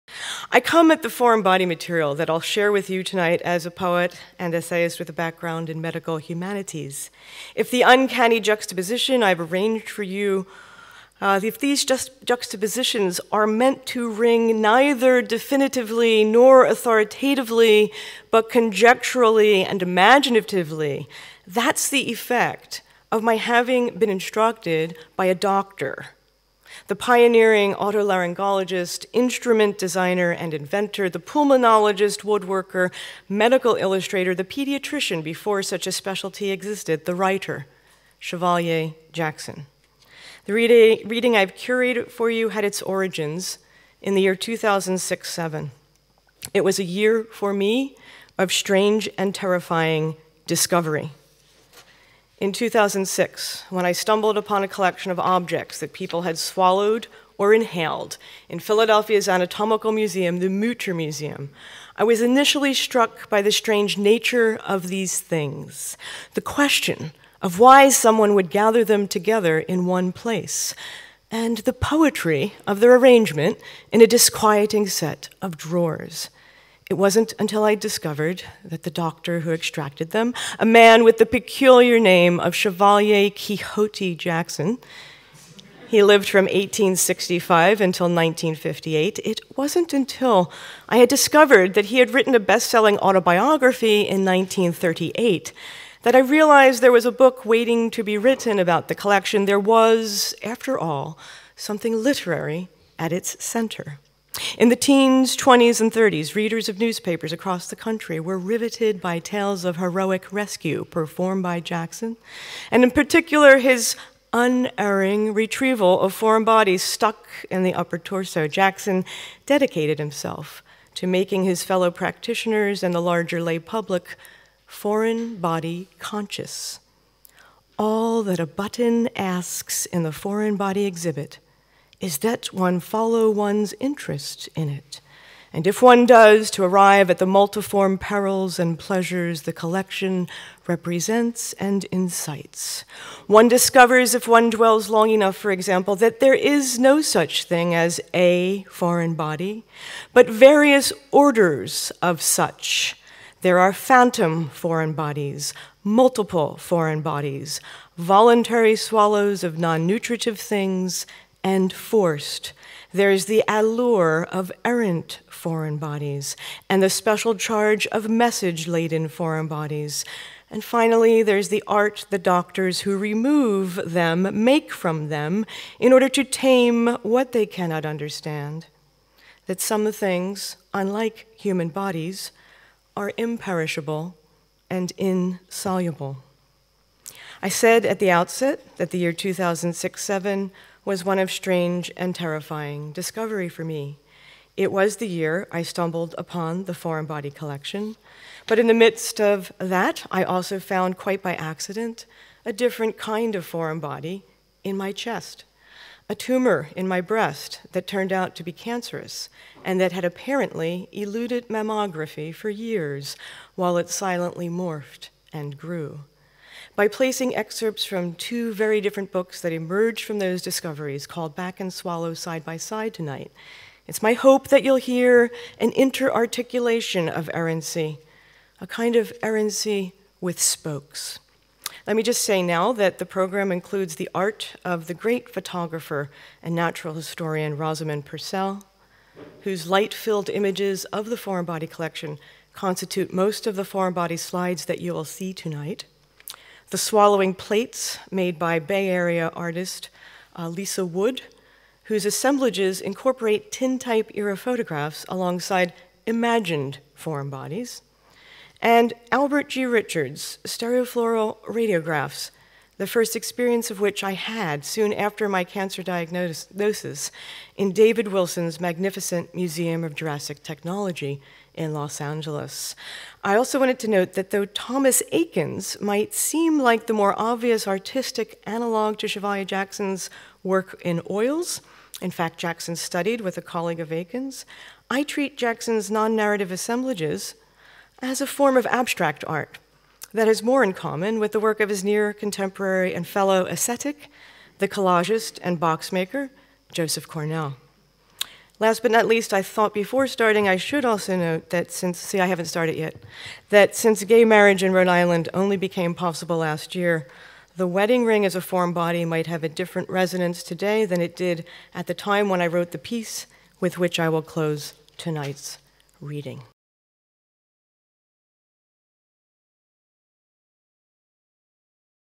Mary Cappello: Errant Foreign Bodies
One of the most popular attractions in Philadelphia’s Mütter Museum is the Chevalier Jackson Foreign Body Collection: a beguiling set of drawers filled with thousands of objects that had been swallowed or inhaled, then extracted non-surgically by a pioneering laryngologist using rigid instruments of his own design. How do people’s throats, lungs, and stomachs end up filled with inedible things, and what do they become once arranged in Dr. Chevalier Jackson’s aura-laden cabinet? Animating the space between interest and terror, curiosity and dread, Mary Cappello brings her own aesthetic of poetic apposition and wend to bear upon Chevalier Jackson’s methods and designs. This illustrated reading juxtaposes the inadvertent art effected by Jackson’s refusal to allow a foreign body to wander with meditations on Cappello’s queerly promiscuous body inside an MRI machine, or the incitement of cancer’s erratic time.

Mary Cappello is a writer and professor of English and Creative Writing at the University of Rhode Island. Her four books of literary nonfiction include Awkward: A Detour (a Los Angeles Times bestseller), and, following Maya Deren, a ritual in transfigured time, Called Back. Her most recent book, Swallow, emerges from the Chevalier Jackson Foreign Body Collection in Philadelphia’s Mütter Museum. A new suite of lyric essays on ‘mood’ and its affinities with clouds, ‘sonorous envelopes’ and dioramas will appear next fall from University of Chicago Press. A recipient of The Bechtel Prize for Educating the Imagination; the Dorothea Lange-Paul Taylor Prize; and, a Guggenheim Fellowship in Nonfiction, Cappello is a former Fulbright Lecturer at the Gorky Literary Institute (Moscow), and currently Holtzbrink Fellow at the American Academy in Berlin.